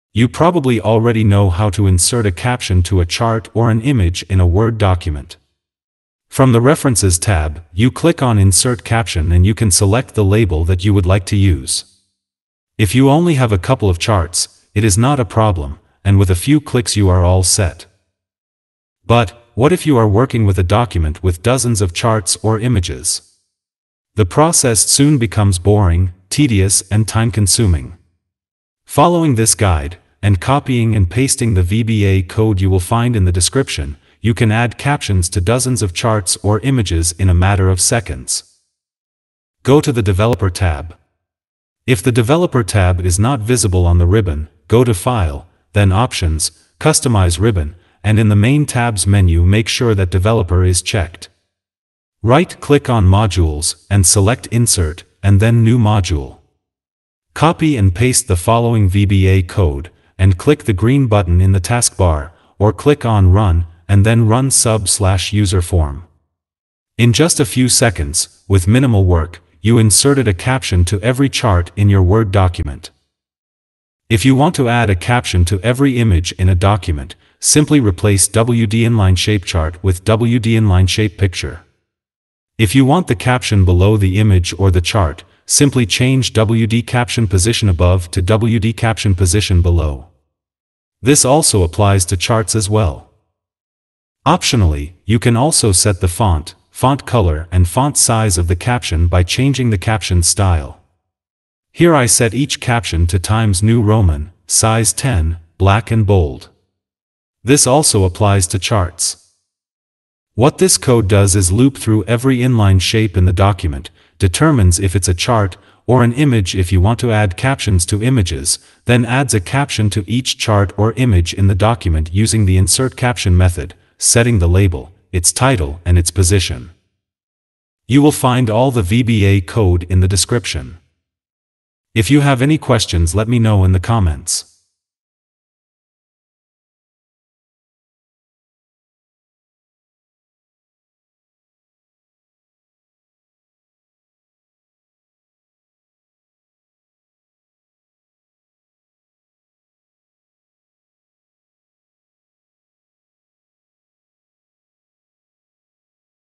Automatically insert captions to every chart or Image in a Word document in seconds - Detailed guide
Automatically insert captions to every chart or Image in a Word document in seconds using some VBA - Detailed guide

You probably already know how to insert a caption to a chart or an image in a Word document. From the References tab, you click on Insert Caption and you can select the label that you would like to use. If you only have a couple of charts, it is not a problem, and with a few clicks you are all set.

But, what if you are working with a document with dozens of charts or images. The process soon becomes boring, tedious and time-consuming. Following this guide, and copying and pasting the VBA code you will find in the description, you can add captions to dozens of charts or images in a matter of seconds!

Go to the developer tab. If the developer tab is not visible on the ribbon, go to File, then Options, Customize Ribbon, and in the Main Tabs menu make sure that Developer is checked. Right click on Modules, and select Insert, and then New Module. Copy and paste the following VBA code, and click the green button in the taskbar, or click on Run, and then Run Sub/Userform. In just a few seconds, with minimal work, you inserted a caption to every chart in your Word document.

If you want to add a caption to every image in a document, simply replace wdInlineShapeChart with wdInlineShapePicture. If you want the caption below the image or the chart, simply change wdCaptionPositionAbove to wdCaptionPositionBelow. This also applies to charts as well.

Optionally, you can also set the font, font color and font size of the caption by changing the captions style. Here I set each caption to Times New Roman, size 10, black and bold. This also applies to charts.

What this code does is loop through every inline shape in the document, determines if it’s a chart, or an image if you want to add captions to images, then adds a caption to each chart or image in the document using the InsertCaption method, setting the Label, its Title and its position.

You will find all the VBA code in the description.

VBA Code to insert a caption to every chart in a Word document

Sub AddCaptions()
Dim intCount As Integer

'loop through inline shapes
For i = 1 To ActiveDocument.InlineShapes.Count
    'check if the current shape is a chart
    If ActiveDocument.InlineShapes.Item(i).Type = _
      wdInlineShapeChart Then
        ''assign a caption to the chart
       ActiveDocument.InlineShapes.Item(i).Select
       Selection.InsertCaption Label:="Figure", _
           Title:=". ", _
           Position:=wdCaptionPositionAbove
        'the above line sets the position of the caption, if you want the caption below the chart change it to wdCaptionPositionAbove
    End If
Next i
            'Optional, remove if you don't want to change the style of the caption
            With ActiveDocument.Styles("Caption").Font
                .Name = "Times New Roman"
                .Size = 10
                .Italic = False
                .Bold = True
                .ColorIndex = wdBlack
            End With
End Sub

VBA Code to insert a caption to every image in a Word document

    'check if the current shape is an image
    If ActiveDocument.InlineShapes.Item(i).Type = _
      wdInlineShapePicture Then
        'insert a caption above the image
       ActiveDocument.InlineShapes.Item(i).Select
       Selection.InsertCaption Label:="Figure", _
           Title:=". ", _
           Position:=wdCaptionPositionAbove
           'the above line sets the position of the caption, change the position to wdCaptionPositionAbove if you want the caption below
    End If

Next i
            With ActiveDocument.Styles("Caption").Font
                .Size = 10
                .Italic = False
                .Bold = True
                .ColorIndex = wdBlack
            End With
End Sub